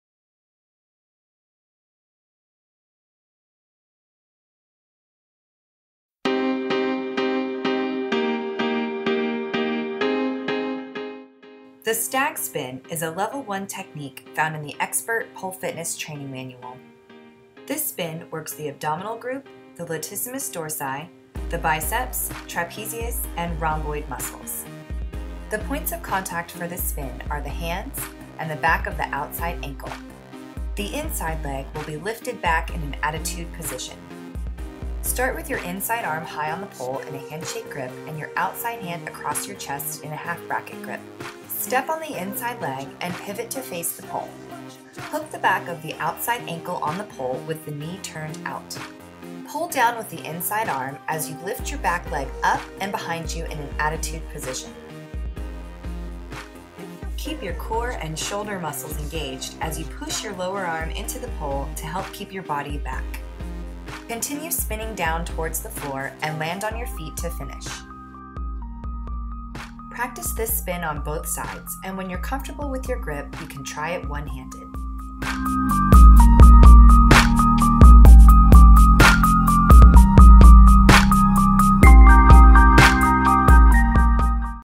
XPERT Tip: The Stag Spin
The XPERT Pole Fitness manual outlines several basic spins in the Level 1 section. This video is a refresher of the Stag Spin. for more info. XPERTPole XPERTtip Music by Peyote Beats